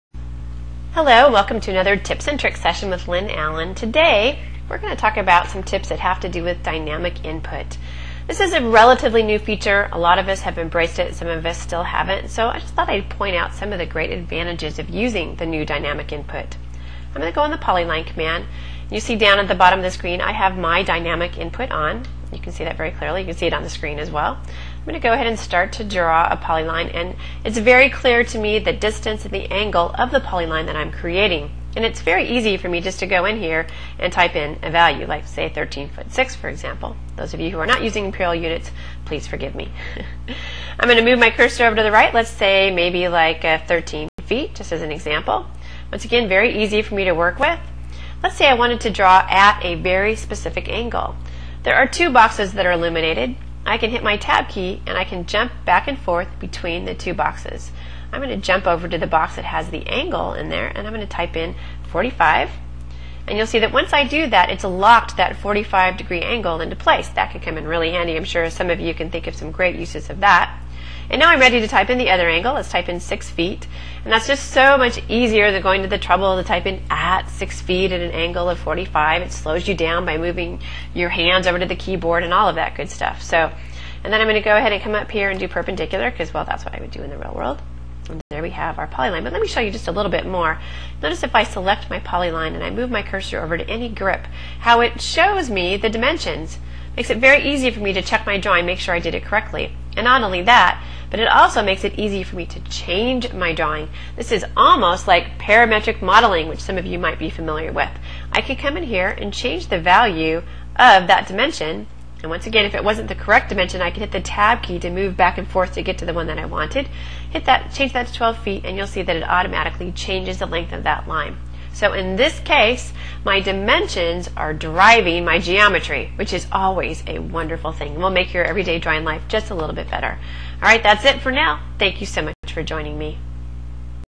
AutoCAD Tip - Make the Most of Dynamic Input (Lynn Allen/Cadalyst Magazine)
These quick tips show a few of the ways you can use AutoCAD's new dynamic input functionality to make your drawing life a little bit easier.